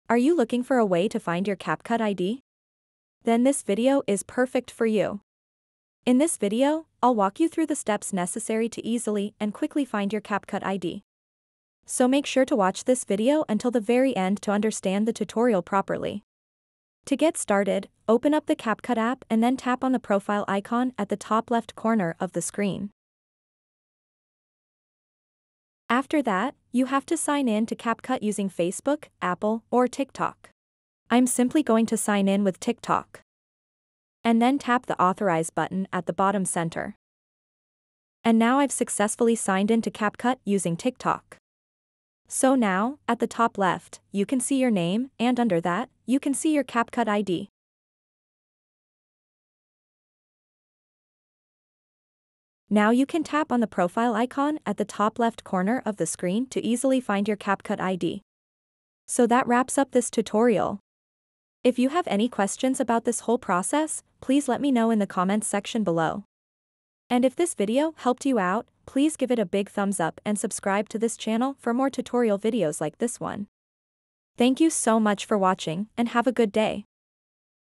How To Find CapCut ID
Are you looking for a way to find your Capcut ID? This video will provide detailed, step-by-step instructions on how to find and access your ID. We'll show you the quickest and easiest way to unlock this information so that you can get back to editing in no time. Come join us as we break down exactly what it takes to find your Capcut ID!